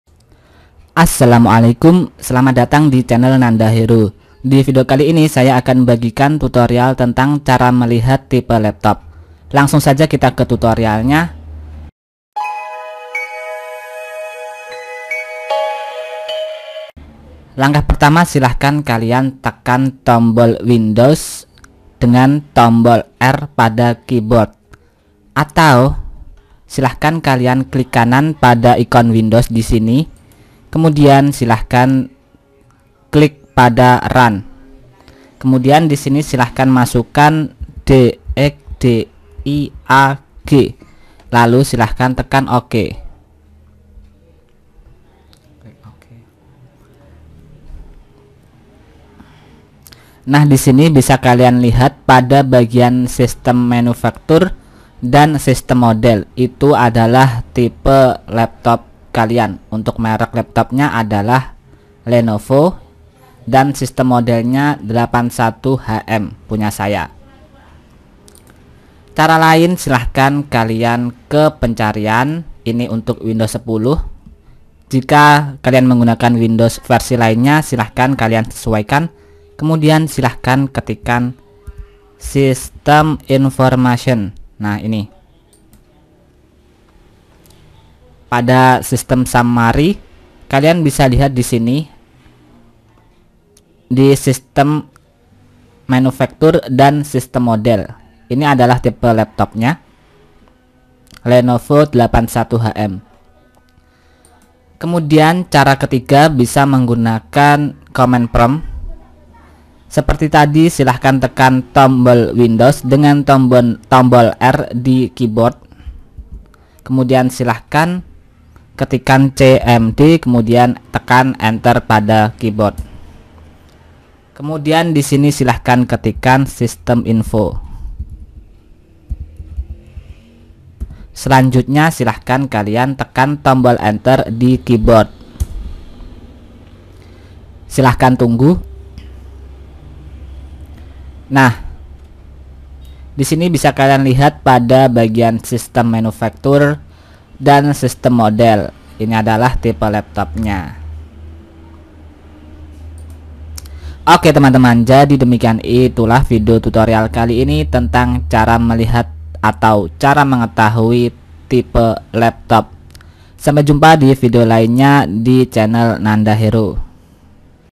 4 CARA MELIHAT TIPE LAPTOP
Tidak tahu tipe laptop yang kamu miliki? Tonton video ini sampai selesai untuk cara melihat tipe laptop dengan mudah.

Ada 4 cara melihat tipe laptop yang saya bagikan, yaitu:
1. Dengan menekan Run kemudian ketikkan dxdiag
2. Melalui sytem information di laptop
3. Melalui CMD
4. Silahkan lihat di Dus Laptop (tidak ada dalam video)